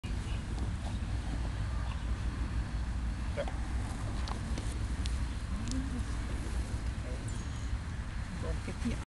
Spelen tijdens de puppycursus
Beagle en kaninchen teckel aan het spelen. Ivm het verschil in gewicht de ene aan de lijn en de ander los. De beagle had al los gespeeld met gelijk formaat honden.